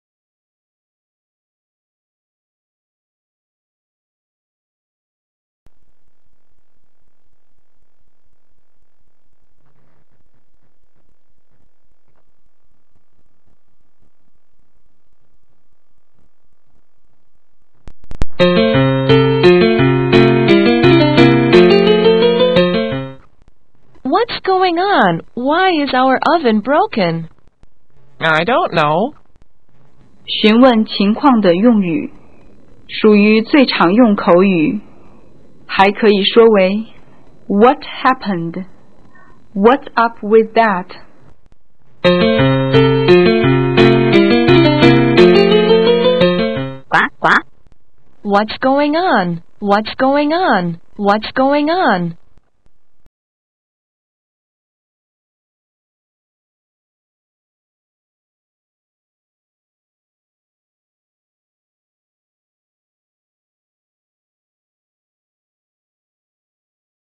English everyday --what's going on 2010.11.17
無敵 英文每日一句
Woody媽媽要做飯的時候發現烤箱壞掉了，很生氣，問Woody"發生什麽事了？"
在英文里，你要怎麼問別人"發生什麽事了？"。在今天的無敵 英文每日一句里會教你怎麼問~